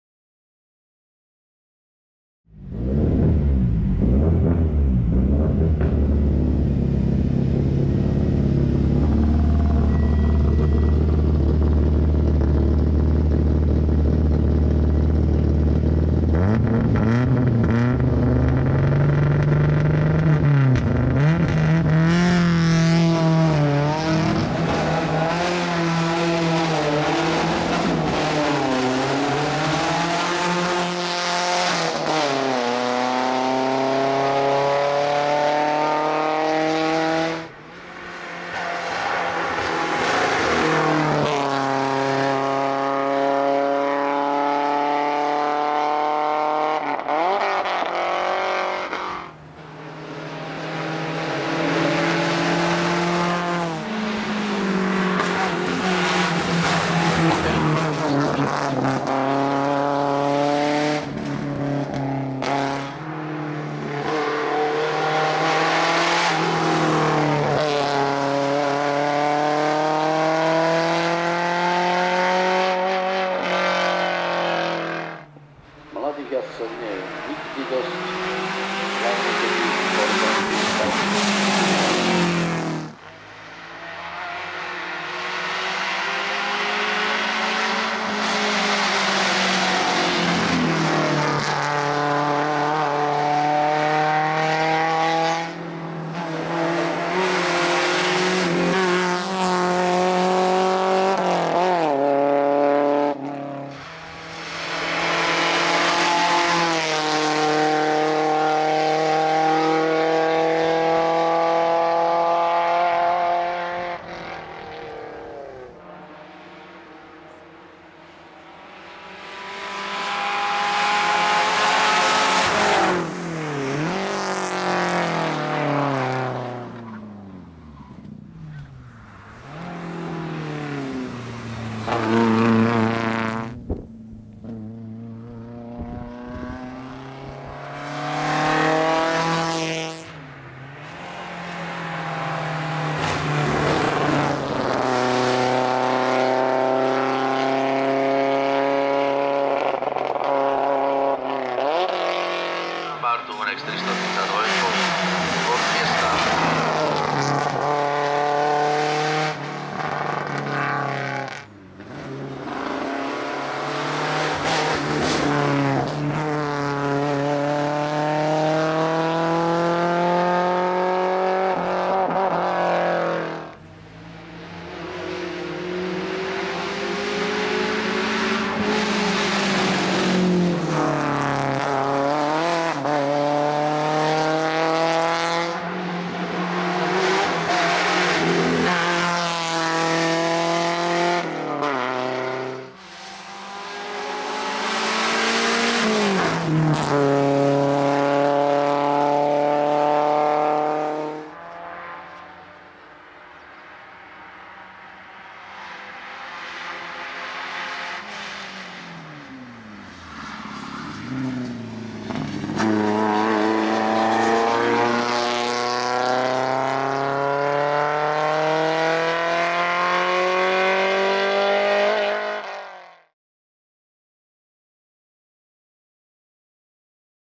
BMW 318is - Štěpa Racing Team - XIV. Divácká 13° - Diváky 2019
Štěpa Racing Team, zastoupený v sobotu jezdcem Milanem Štěpánkem a v neděli jezdcem Mirkem Novotným, se představil na XIV. Divácké 13° roku 2019 s vozem BMW 318is, kde si prvně jmenovaný Milan Štěpánek dojel v sobotu pro bronz ve třídě HA1-2000.